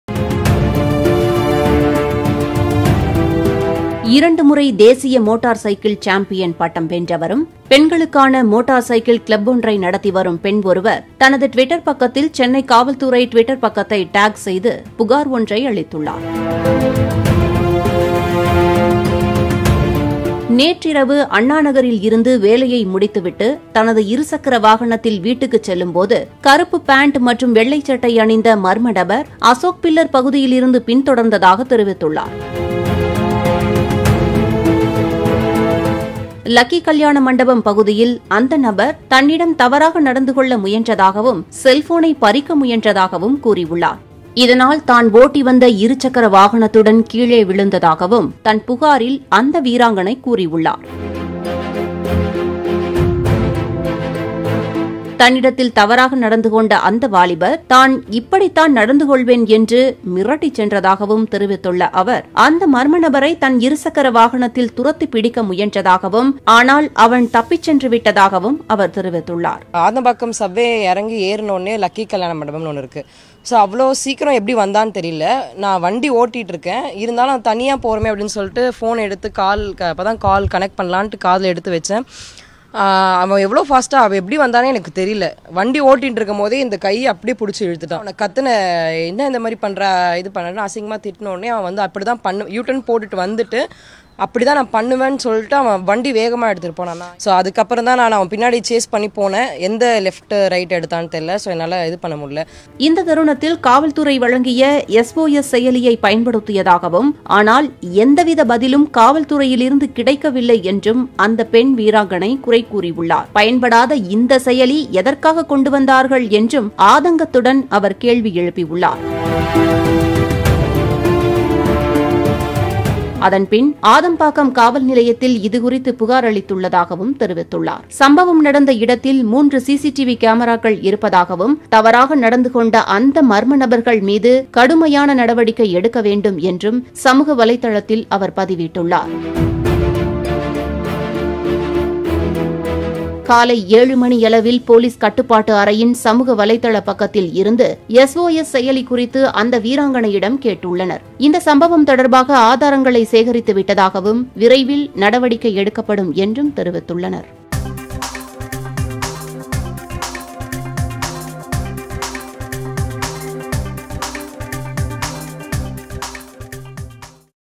’இப்படித்தான் நடப்பேன்’ - வாலிபரின் பகீரங்க சவால்.. பைக்ரேஸ் சாம்பியனிடத்தில் அத்துமீறல்..!
தேசிய சாம்பியனிடத்தில்  இரவில்  அட்டூழியம்.. இப்படித்தான் நடப்பேன் என்றும் சவாடல்.. செயல்படாத எஸ் ஓ எஸ் கருவி